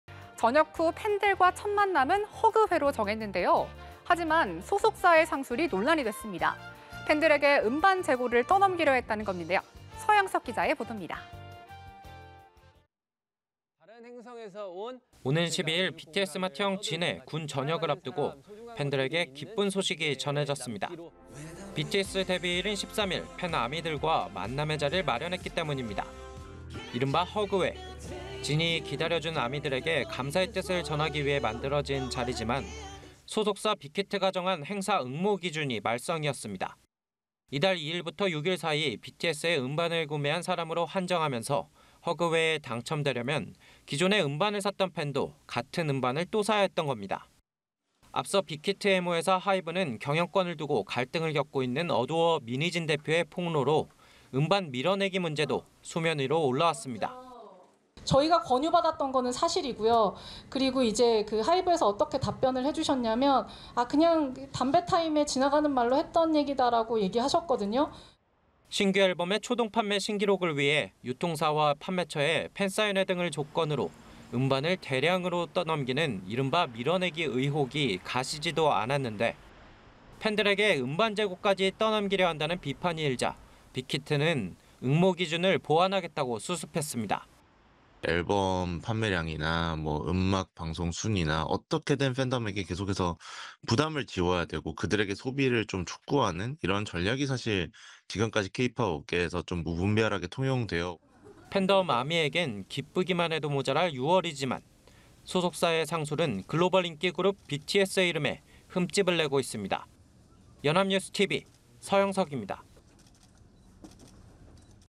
BTS 진 보려면 재고 음반 사라…하이브 상술 논란 / 연합뉴스TV (YonhapnewsTV)
BTS 진 보려면 재고 음반 사라…하이브 상술 논란

[앵커]

전역 후 팬들과 첫 만남은 '허그회'로 정했는데요.

하지만 소속사의 상술이 논란이 됐습니다.

팬들에게 음반 재고를 떠넘기려 했다는 겁니다.

서형석 기자입니다.

[기자]

오는 12일 BTS 맏형 진의 군대 전역을 앞두고 팬들에게 기쁜 소식이 전해졌습니다.

BTS 데뷔일인 13일 팬, 아미들과 만남의 자리를 마련했기 때문입니다.

이른바 '허그회', 진이 기다려준 아미들에게 감사의 뜻을 전하기 위해 만들어진 자리지만 소속사 빅히트가 정한 행사 응모 기준이 말썽이었습니다.

이달 2일부터 6일 사이 BTS의 음반을 구매한 사람으로 한정하면서, 허그회에 당첨되려면 기존에 음반을 샀던 팬도 같은 음반을 또 사야 했던 겁니다.

앞서 빅히트의 모회사 하이브는 경영권을 두고 갈등을 겪고 있는 어도어 민희진 대표의 폭로로 음반 '밀어내기' 문제도 수면 위로 올라왔습니다.

[민희진 / 어도어 대표 (지난달 31일)] "저희가 (음반 밀어내기를) 권유받았던 거는 사실이고요. 그리고 이제 하이브에서 어떻게 답변을 해 주셨냐면 그냥 '담배타임'에 지나가는 말로 했던 얘기라고 하셨거든요"

신규 앨범의 초동 판매 신기록을 위해 유통사와 판매처에 팬사인회 등을 조건으로 음반을 대량으로 떠넘기는 이른바 '밀어내기' 의혹이 가시지도 않았는데, 팬들에게 음반 재고까지 떠넘기려 한다는 비판이 일자 빅히트는 응모 기준을 보완하겠다고 수습했습니다.

[김도헌 / 음악평론가] "앨범 판매량이나 음악 방송 순위나 어떻게든 팬덤에게 계속해서 부담을 지우게 하고 소비를 촉구하는 이런 전략이 지금까지 K팝 업계에서 무분별하게 통용…"

팬덤 아미에겐 기쁘기만 해도 모자랄 6월이지만, 소속사의 상술은 글로벌 인기 그룹 BTS의 이름에 흠집을 내고 있습니다.

(끝)